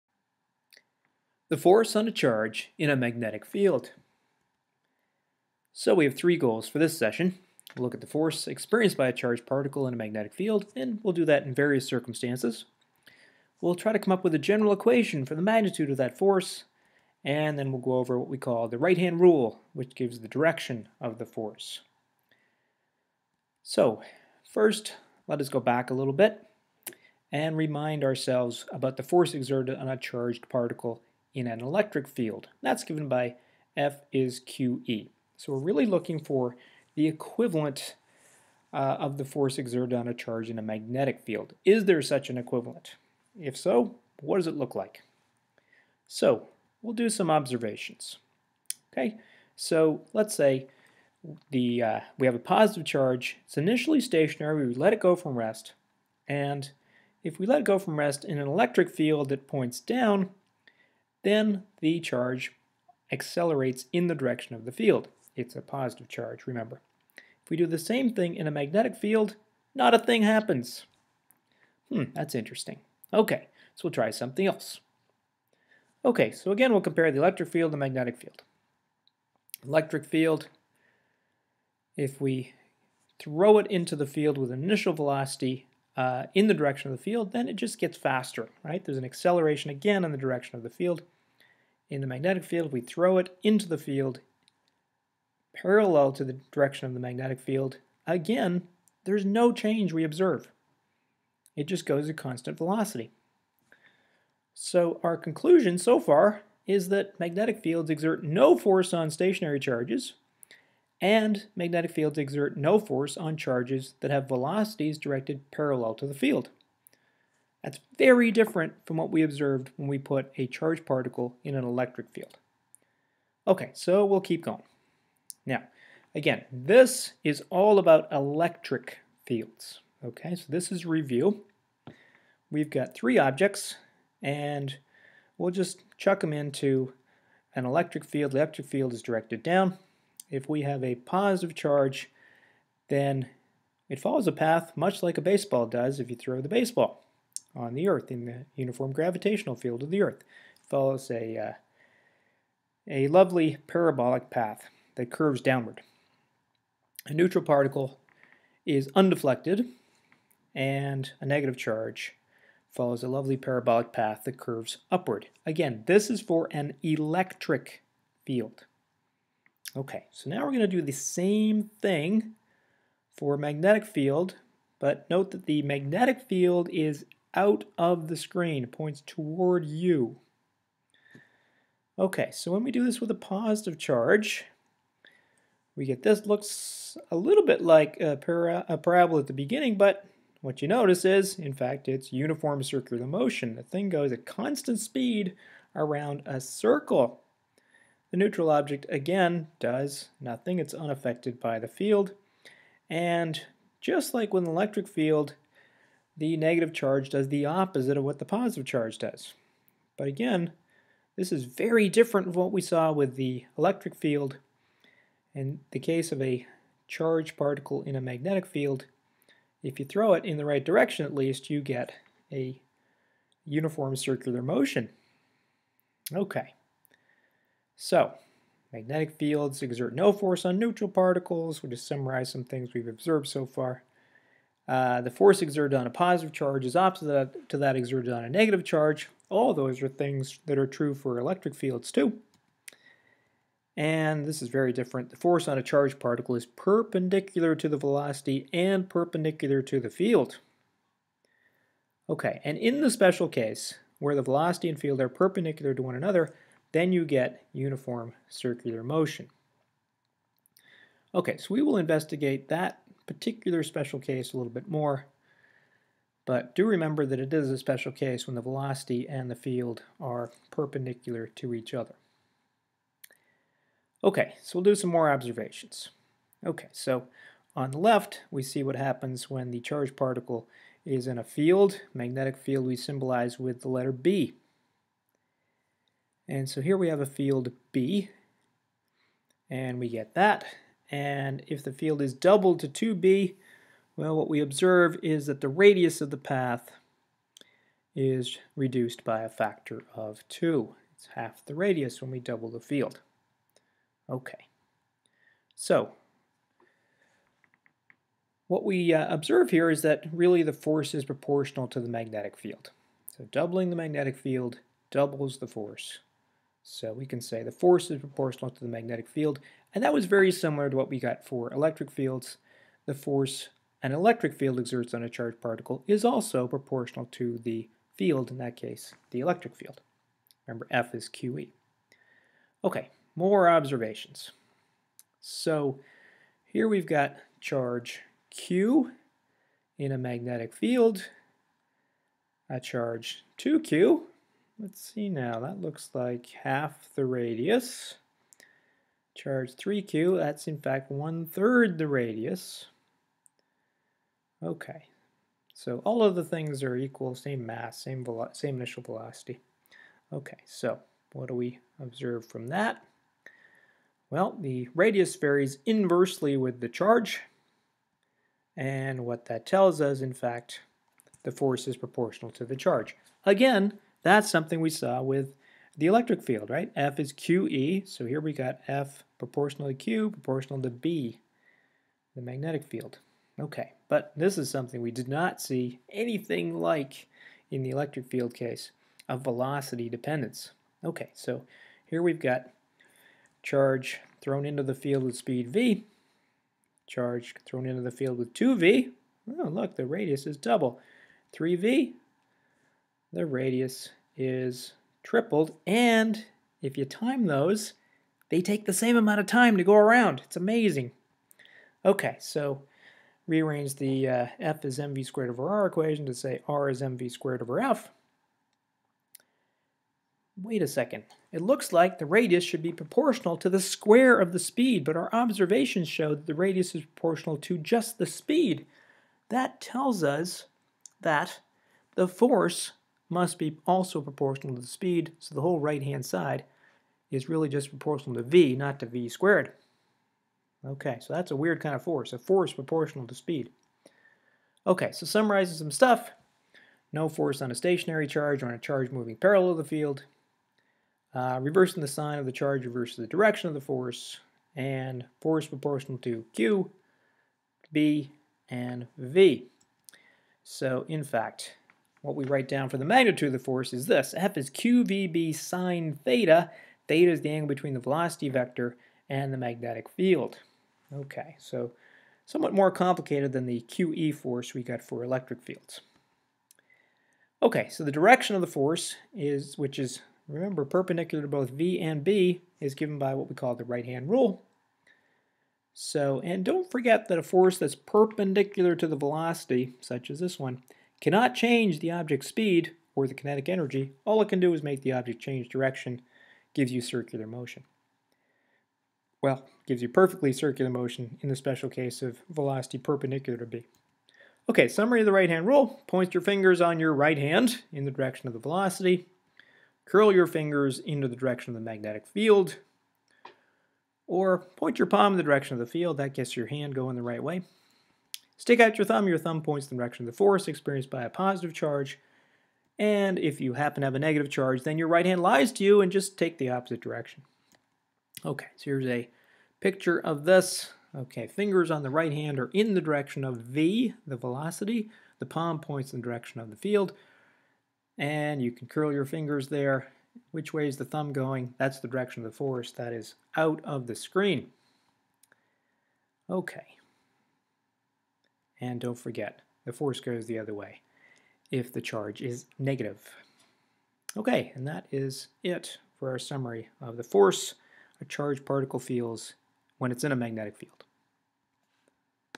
The force on a charge in a magnetic field
In this video, we talk about the force applied on a charged particle by a magnetic field. There are some similarities between this and the force applied by an electric field, but there are also important differences, and these differences bring us back to uniform circular motion.